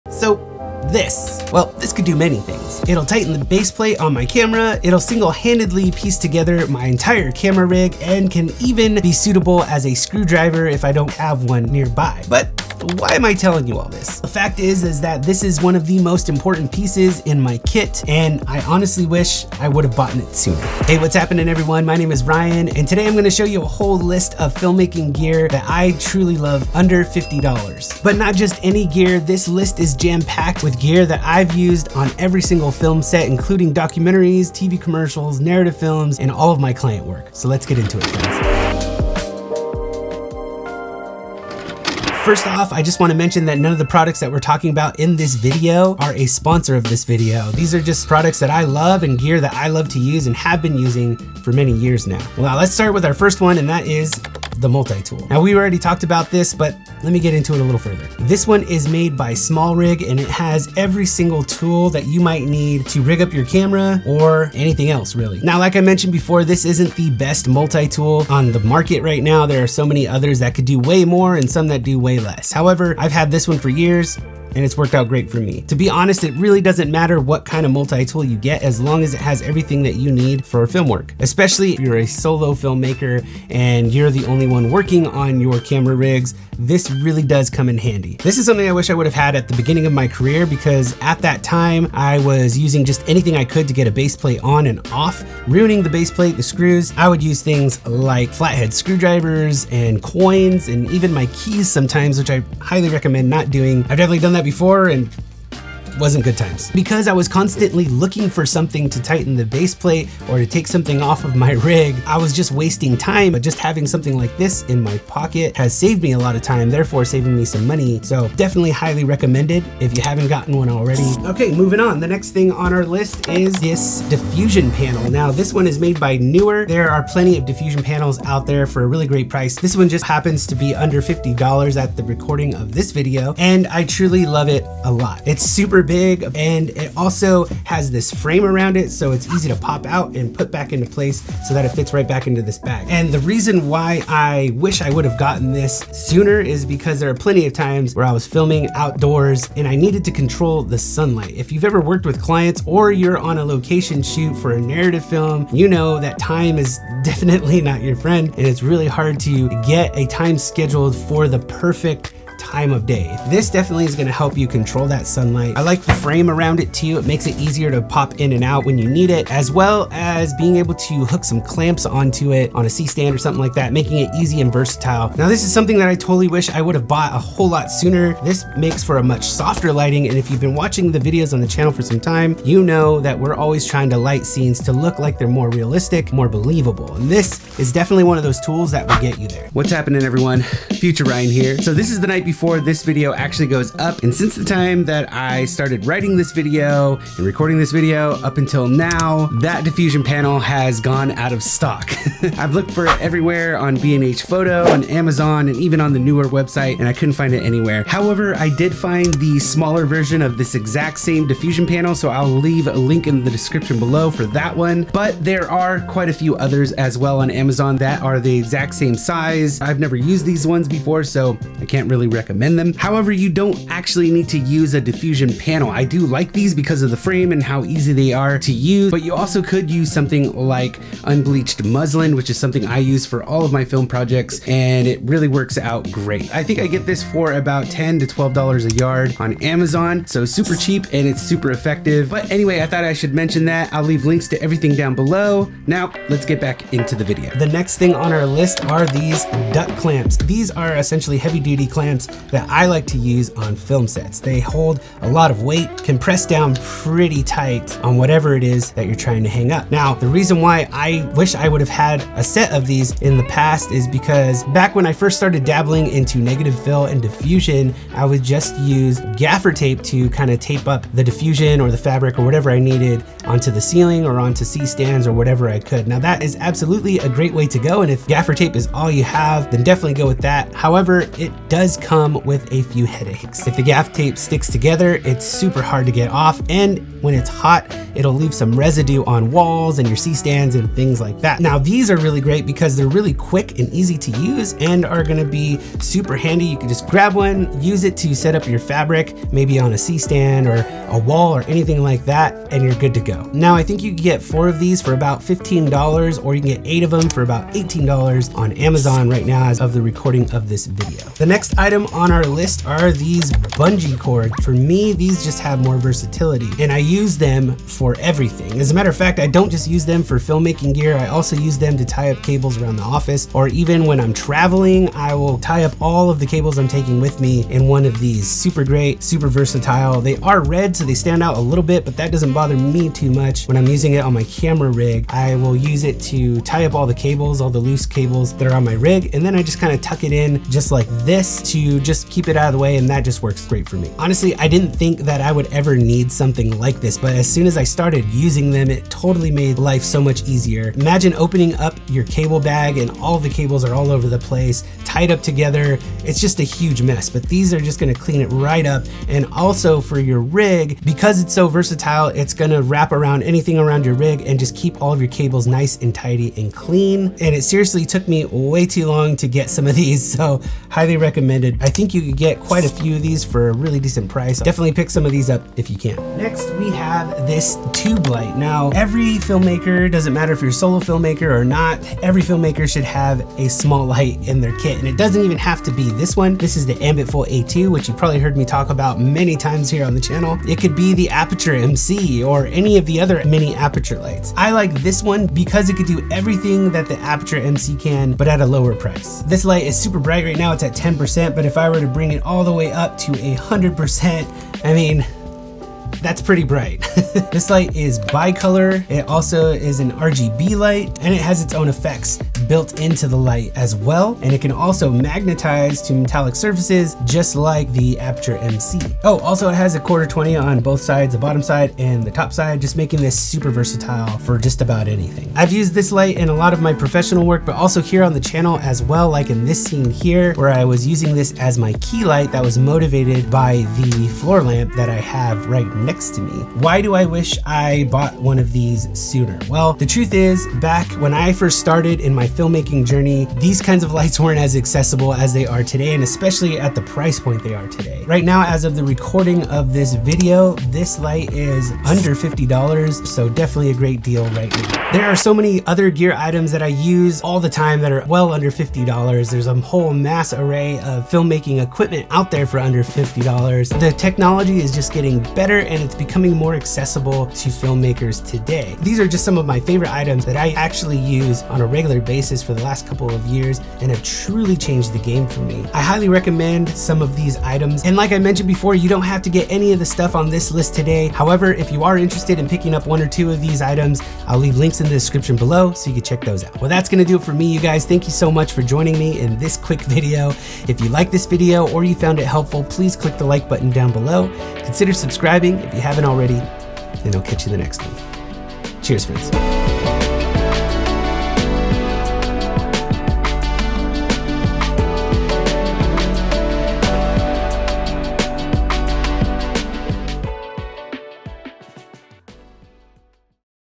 Filmmaking Gear I Wish I Bought Sooner: UNDER $50
I've compiled a list of filmmaking gear that I genuinely love, all under $50. These tools have become my trusted companions, used consistently in nearly every film project. This is the kind of gear I wish I had discovered sooner in my filmmaking journey. Can you relate? I would love to connect!

Gear Mentioned In This Video:
1. Multitool
2. Diffusion
- Diffusion Panel
- White Muslin Diffusion
3. Duck Clamps
4. Bungee Ties
5. Tube Light

Cameras I Used In This Video
Sony FX3
Sony A7IV
Sony 24-70mm 2.8 GM
Sony 35mm 1.8